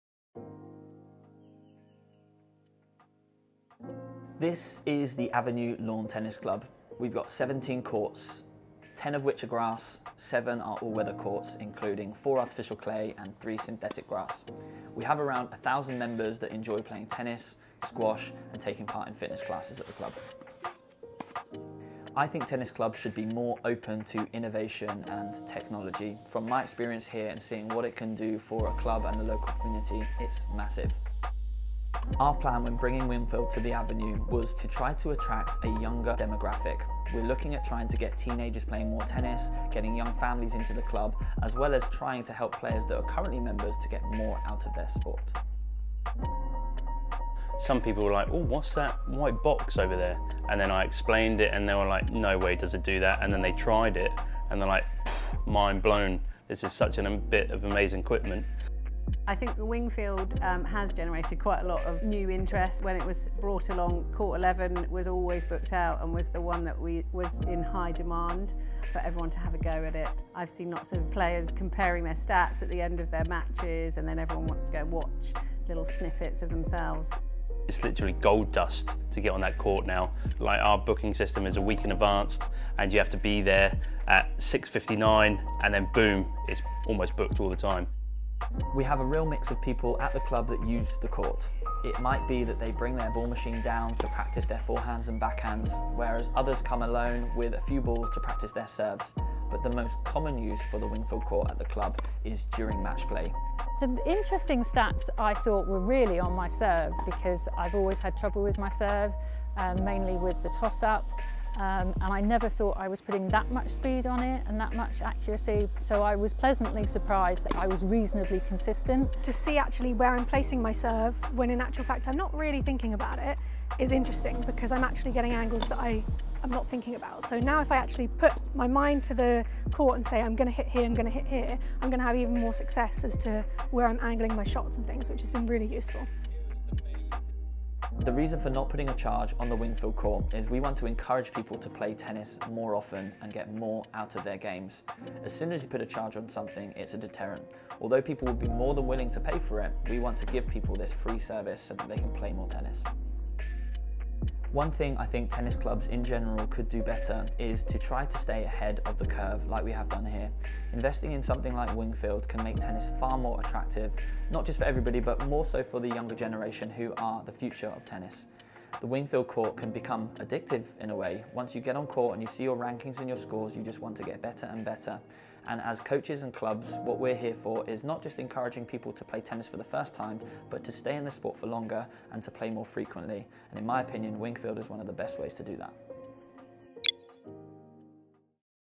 Wingfield x The Avenue Tennis
The Wingfield club series introduces outstanding Wingfield clubs showcasing best practices of how to integrated Wingfield in club life, playing and
 training operations.

The Avenue Tennis tried to attract a younger demographic with Wingfield. People were looking at new ways to get teenagers playing more tennis, getting young families into the club as well as trying to help existing members to get more out of their sport. Let's see how it went...

🎥  by Bass Films